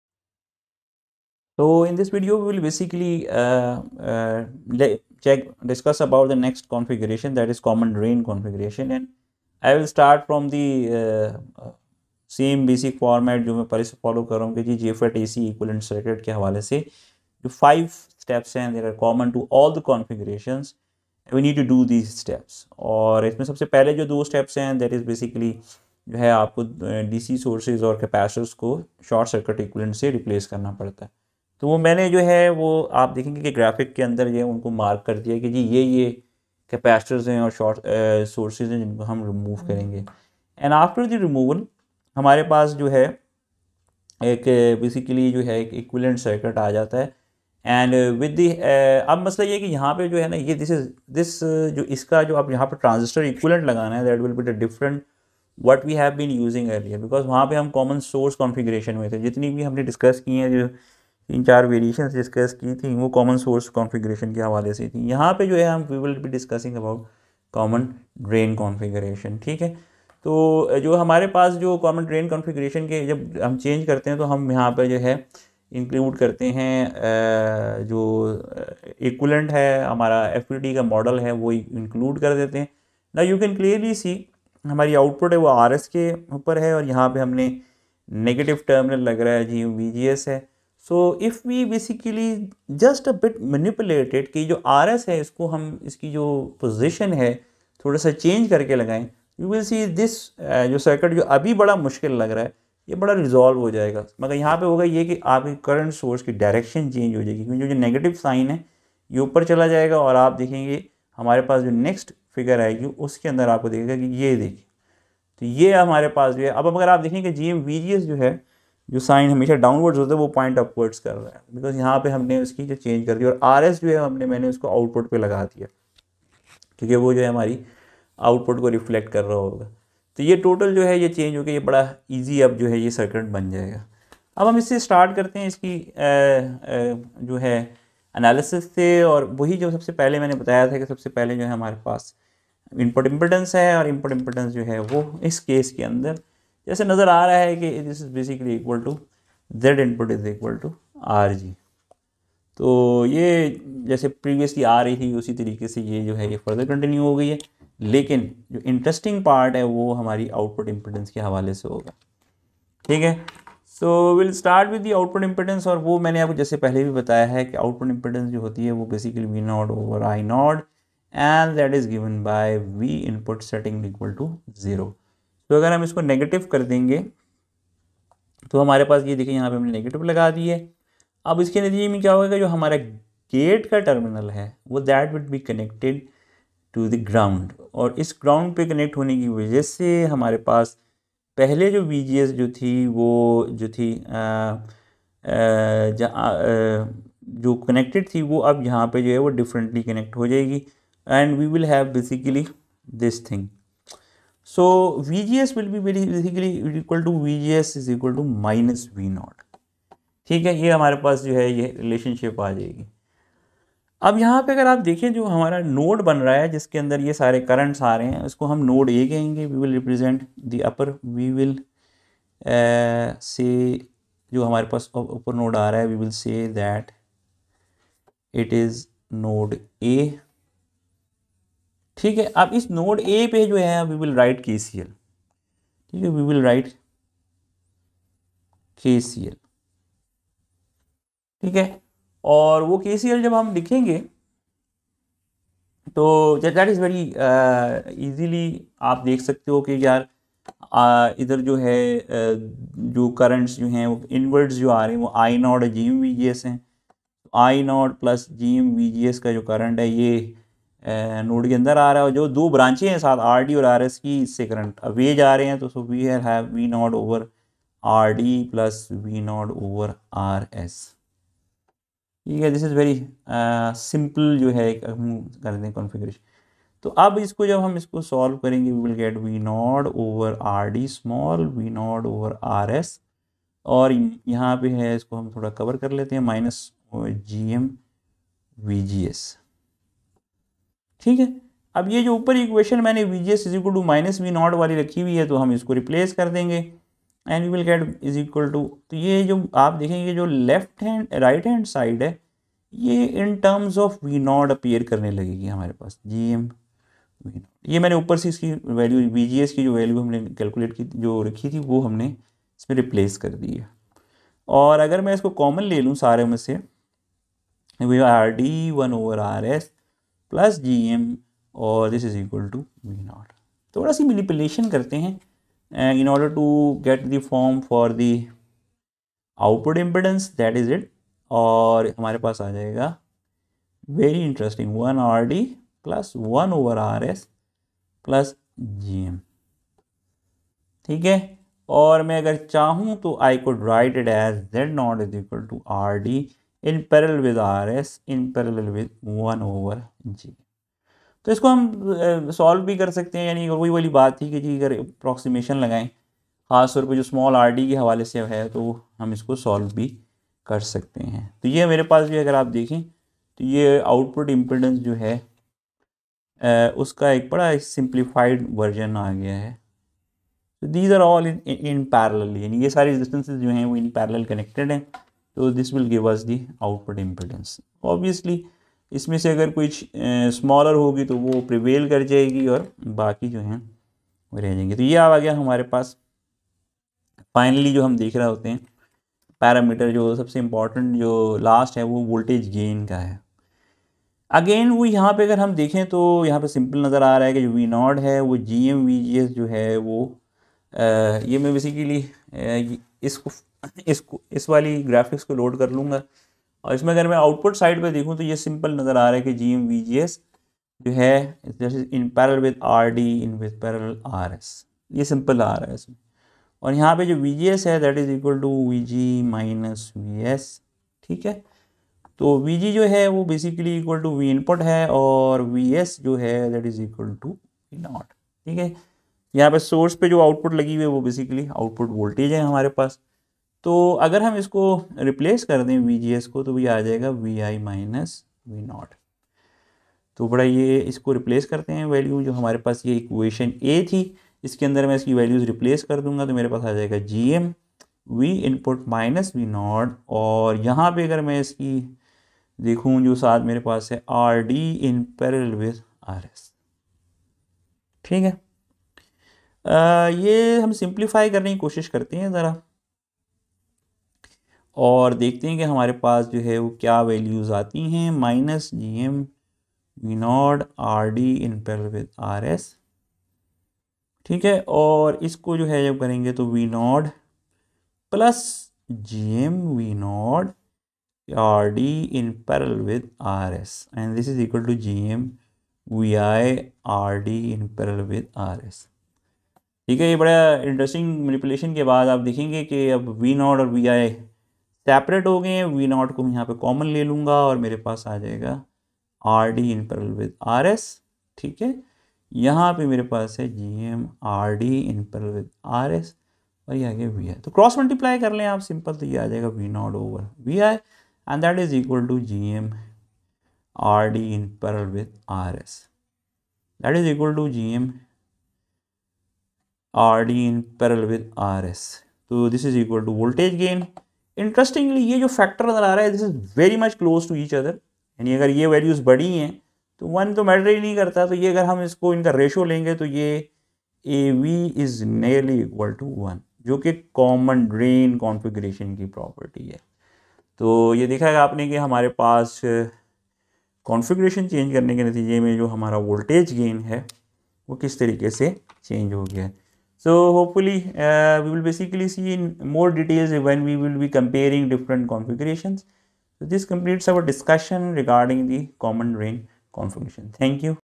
JFET Common Drain Configuration
This video provides detailed AC analysis of Common Drain Configuration. It includes calculations for input impedance, output impedance and voltage gain.